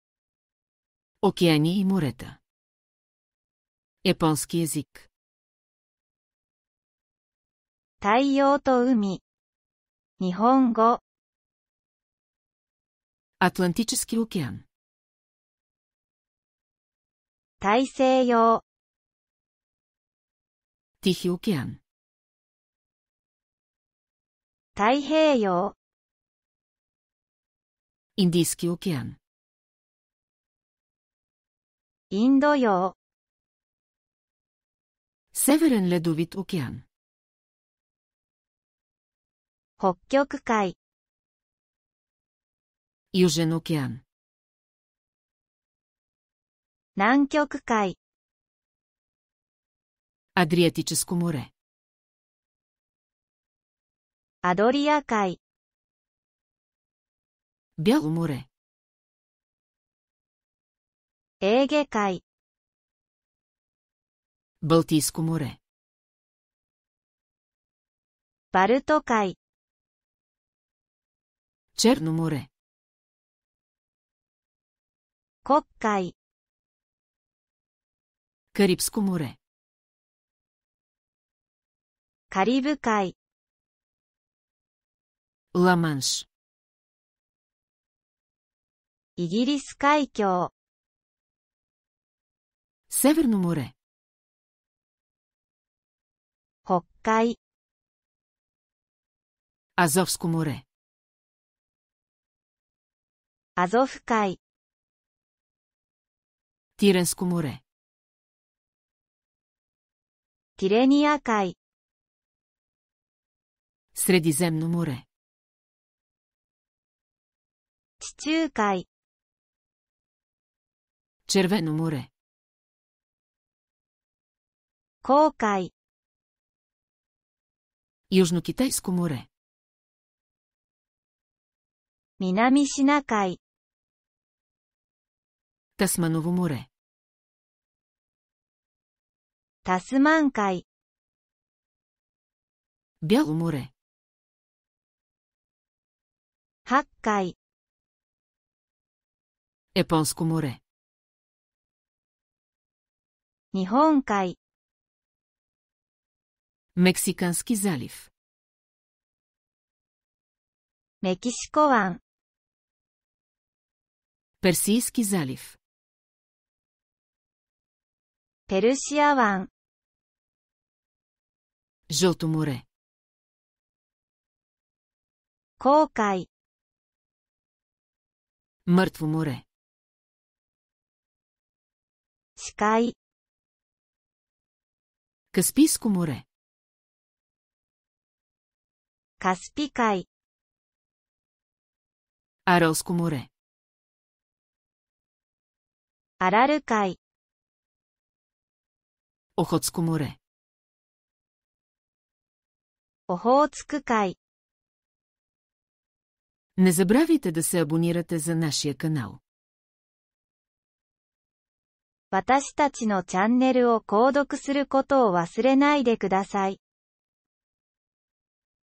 Океани и морета (Японски език) (bg-ja)
Fone TV
bg-ja-31
大洋と海 (日本語)
Атлантически океан - 大西洋
Тихи океан - 太平洋
Индийски океан - インド洋
Северен ледовит океан - 北極海
Южен океан - 南極海
Адриатическо море - アドリア海
Бяло море - エーゲ海
Балтийско море - バルト海
Черно море - 黒海
Карибско море - カリブ海
Ла Манш - イギリス海峡
Северно море - 北海
Азовско море - アゾフ海
Тиренско море - ティレニア海
Средиземно море - 地中海
Червено море - 紅海
Южнокитайско море - 南シナ海
Тасманово море - タスマン海
Бяло море - 白海
Японско море - 日本海
Мексикански залив - メキシコ湾
Персийски залив - ペルシア湾
Жълто море - 黄海
Мъртво море - 死海
Каспийско море - カスピ海
Аралско море - アラル海
Охотско море - オホーツク海